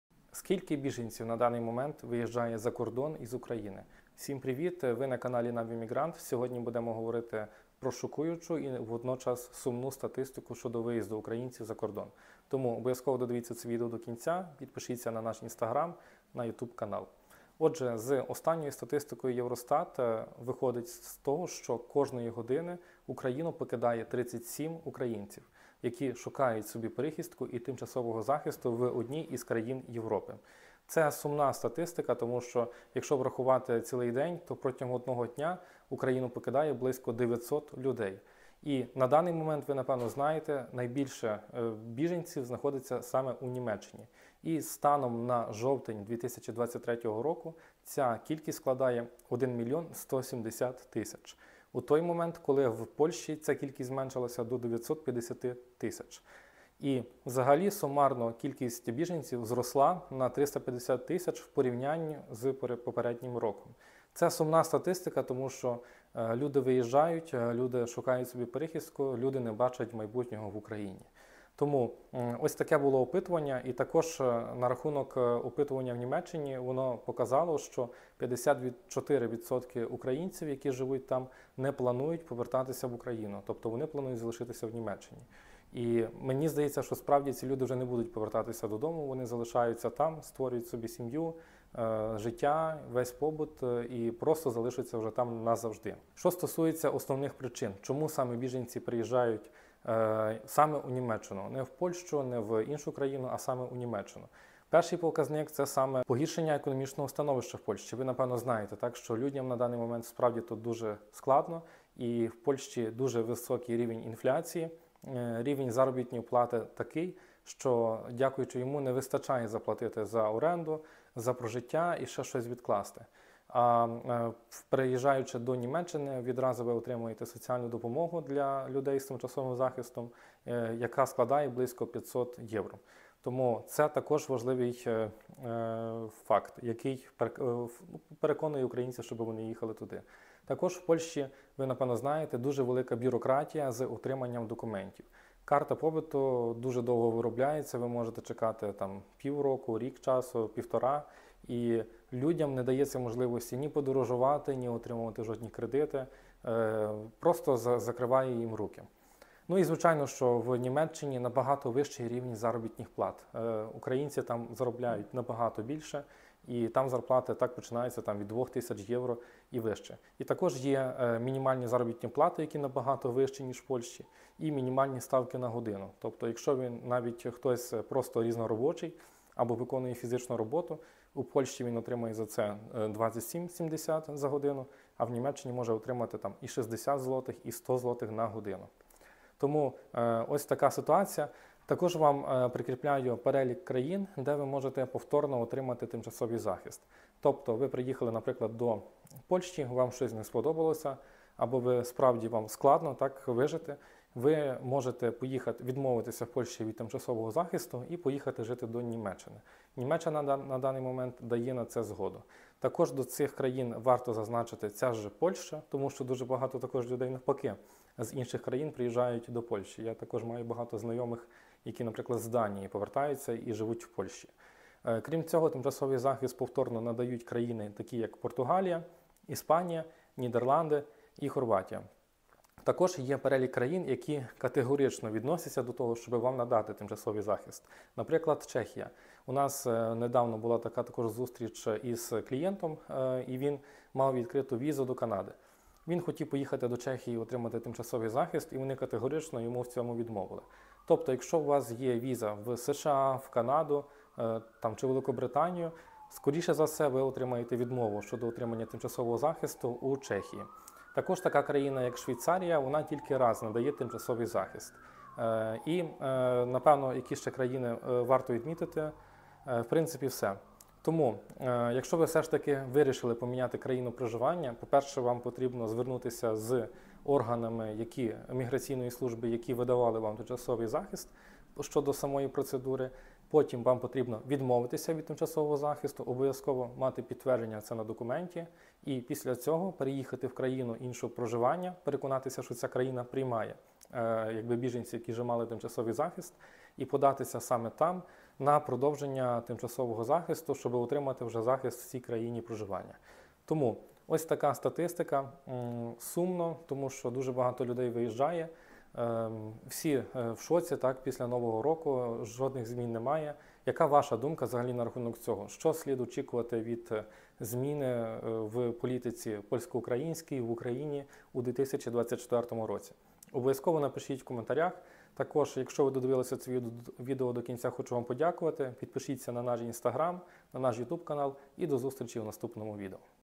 Скільки біженців на даний момент виїжджає з України? Основні причини, статистика.
Телеграм з корисною інформаціює: Мій приватний курс Корисна інформація для українців. Скільки біженців виїжджає з України? Шокуюча і водночас сумна статистика.

Згідно зі статистикою Eurostat:
-кожної години 37 українців виїжджає за кордон через війну в пошуках кращого майбутнього. Тобто кожного дня практично 900 людей покидає Україну.

На даний момент зі статусом тимчасового захисту у Європі зареєстровано:
4 млн 160 тис осіб українських біженців.

Протягом 2023 року до Європи приїхало на 320.000 біженців більше, ніж за попередній рік.

Найбільше біженців зареєстровано у Німеччині, де на початок жовтня 2023 року перебуває близько 1 млн 170 тис українських біженців з тимчасовим захистом на підставі §24.

коли у Польщі кількість зменшилась до 957 тис.

Останнє опитування в Німеччині
показало, що 45% українських біженців мають намір залишитися в країні на кілька років або назавжди.

Також показую статистику країн ЄС, в яких перебувають наші біженці:
Німеччина: 1 млн. 170 тис
Польща 957 тис.
Чехія 361 тис.
Італія 170 тис.
Франція 100 тис.
Естонія 80 тис.
Болгарія 80 тис.
Литва 40 тис.
Австрія 50 тис.
Швеція 40 тис.
Данія 40 тис.

Ось основні причини, чому біженці вибирають саме Німеччину як країну прихистку:

- Погіршення економічного становища у Польщі, людям не вистачає зарплати на
прожиття,
-Інфляція, підвищення цін на житло, і
харчування;
 ⁃ Негативне ставлення і дискримінація українців;
 ⁃ Довга бюрократія для отримання документів і легалізації;
 ⁃ в Німеччині можна заробляти значно більше від декількох тисяч євро на місяць;

Також показую список країн, де ви можете поїхати, навіть якщо отримали тимчасовий захист в іншій країні:

1. Німеччина:
Ця країна надалі продовжує надавати
тимчасовий захист не зважаючи на захист
іншої країни.

2. Польща
3 практики більшість прохань щодо
надання повторного тимчасового захисту мають позитивний результат.
Але рішення про надання статусу залишається на дискреційній, тобто індивідуальній основі уженду.

3. Інші популярні країни, які надають
повторний захист це:
 ⁃ Португалія
 ⁃ Іспанія
 ⁃ Нідерланди
 ⁃ Хорватія

Ось список країн, які категорично відносяться до повторного надання тимчасового захисту:

Чехія: вам можуть відмовити у захисті якщо у Вас є віза до Канади чи США, або ж якщо у Вас є часовий захист іншої країни.

Норвегія надає часовий захист лише раз.
У Швейцарії ви не зможете отримати тимчасовий захист, якщо ви вже мали його в
іншій країні.

Якщо ви все ж таки вирішили поміняти країну тимчасового захисту радимо:

 1. Звʼязатися з міграційними органами країни, до якої плануєте переїхати.
 2. Відмовитись від тимчасового захисту країні, де ви перебуваєте і отримати офіційне підтвердження.